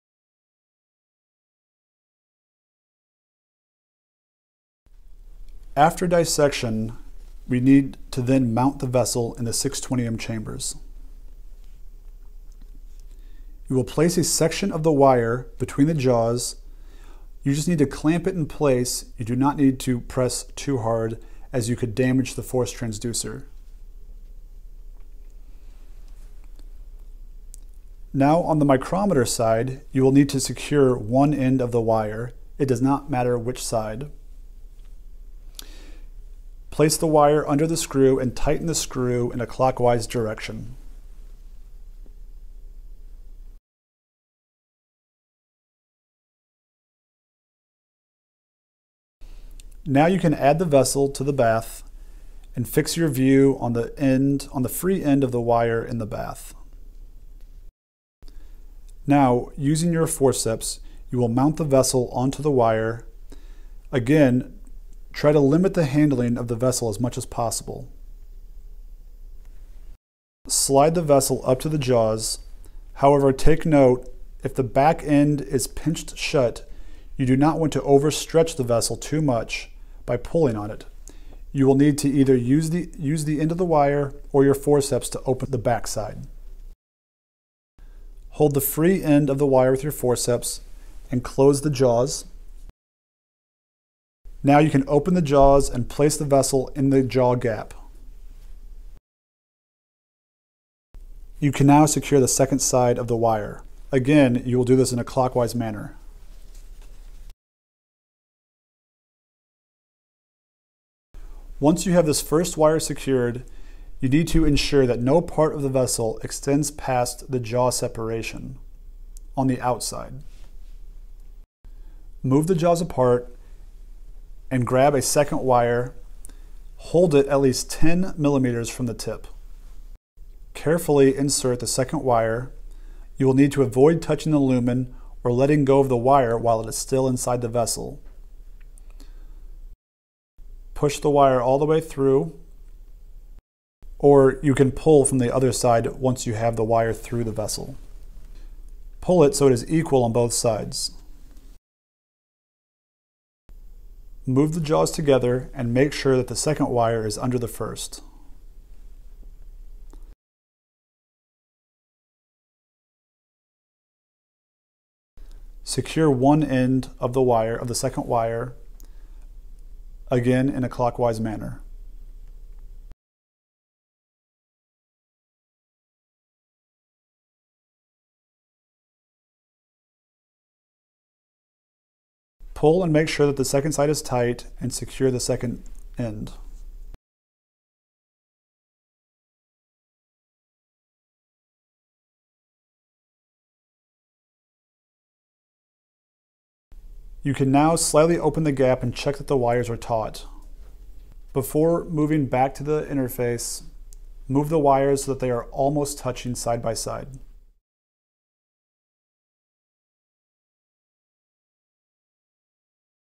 How To | Mount a Mesenteric Artery | 620M
In this video, you will be learning about how to mount a mesenteric artery using a DMT wire myograph - 620M.